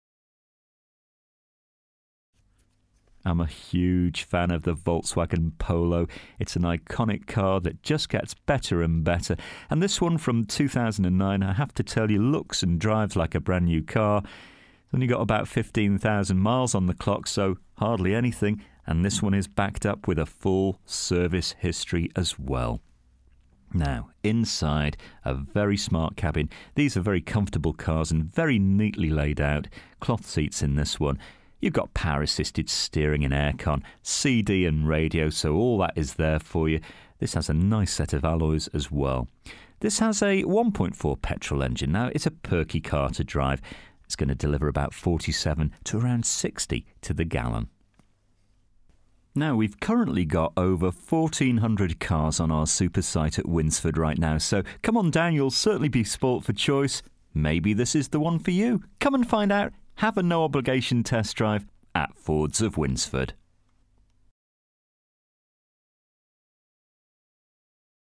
VOLKSWAGEN POLO HATCHBACK (2009) 1.4 SEL 5DR - YF59DUV
DETAILS:
First registered: 22/12/2009
Current mileage: 15170
Registration: YF59DUV
Colour: SILVER

FEATURES:
full service history, all round electric windows, remote central locking, cloth interior, all round airbags, air conditioning, pas, abs, alloy wheels, metallic paint, cd, traction control, parcel shelf present, hand book presen

This used car is for sale at Fords of Winsford Car Supermarket in Cheshire, UK